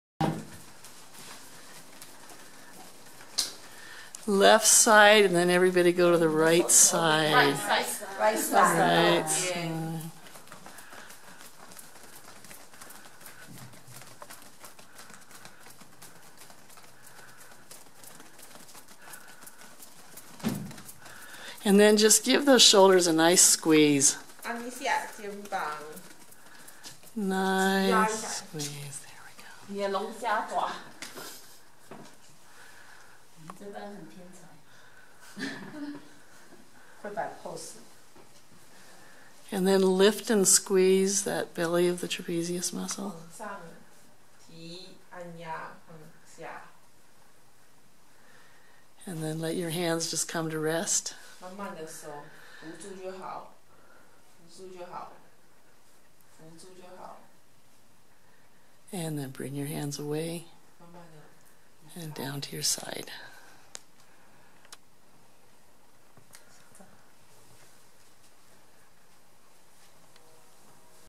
"Percussion line" - Comfort Touch Nurturing Acupressure
Students from Taiwan study Comfort Touch (nurturing acupressure for the elderly and the ill) with Mary Kathleen Rose in Longmont, Colorado. Here they take a moment to practice technique on the shoulder well.